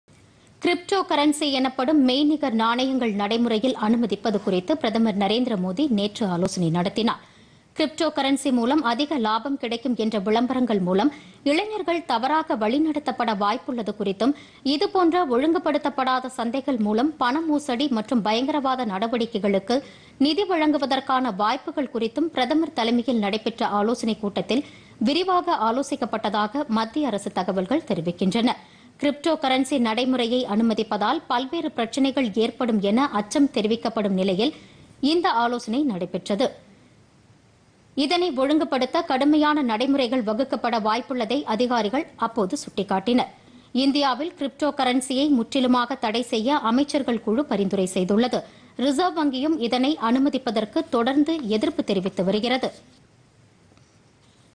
கிரிப்டோ கரன்சி- பிரதமர் ஆலோசனைPodhigaiTamilNews
கிரிப்டோ கரன்சி நடைமுறை  குறித்து பிரதமர் நரேந்திர மோதி  தலைமையில்  புதுதில்லியில் ஆலோசனை